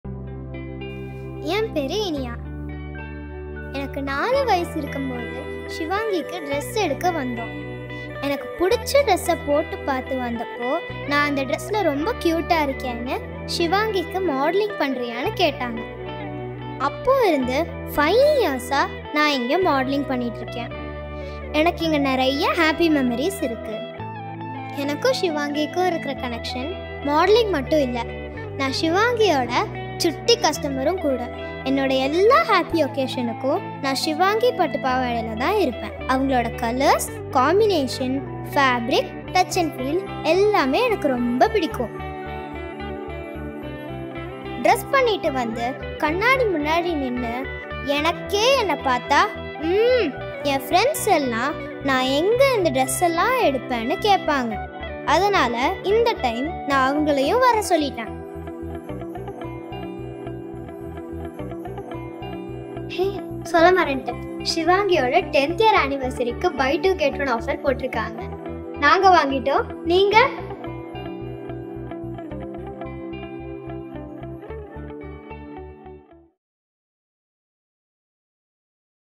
"Celebrating 10 Years of Tradition & Elegance | Shivangi Clothing Anniversary Special 🌸" Buy 2 Get 1
✨ Celebrating a Decade of Tradition and Elegance!

From a humble beginning in the heart of South India, Shivangi Clothing has been dedicated to revamping the traditional Pattupavadai, bringing its timeless beauty to the modern world. Our journey started with a passion for preserving our rich heritage while adding a touch of contemporary flair. Over the past 10 years, we’ve grown from a small business to a beloved brand, reaching thousands of people worldwide who cherish the elegance of our handcrafted designs.

As we step into our 10th year in the retail business, we extend our heartfelt gratitude to our loyal customers, our dedicated team, and especially our adorable little clients and models whose charm and grace have showcased our work to so many. Your support has been invaluable throughout our journey. Thank you for being a part of our story!

To commemorate our 10th anniversary, we are thrilled to offer exclusive discounts as a token of our appreciation.

Visit our stores in Chinniampalayam and Ramnagar in Coimbatore to avail these special offers.